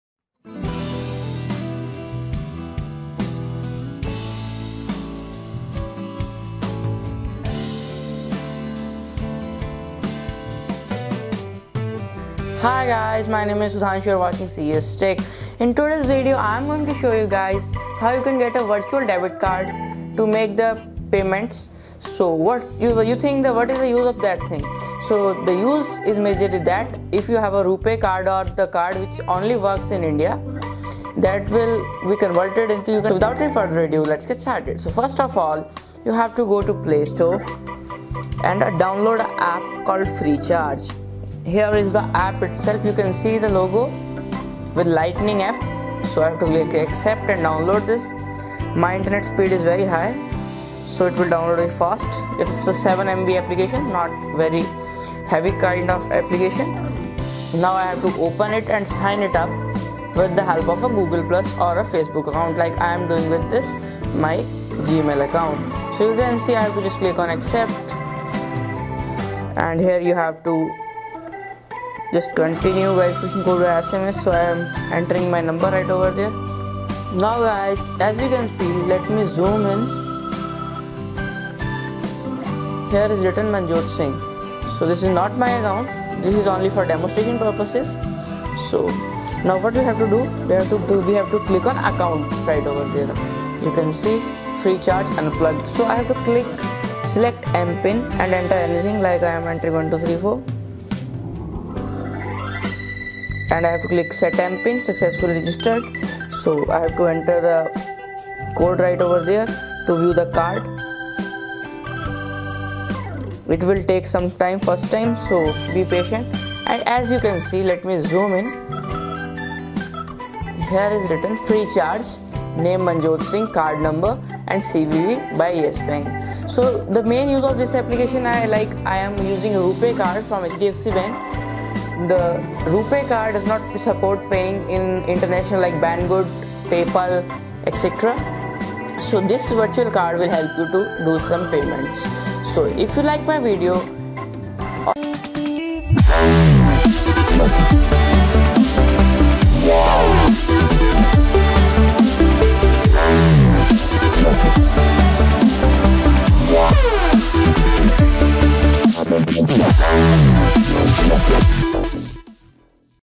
get debit card for international payments : )..
Generate your FreeCharge GO MasterCard instantly and shop online in all the leading Indian e-commerce sites. FreeCharge is the fastest way to pay. To know how, watch this video!

channel update now all vdo in hindi now : ) xd My Gear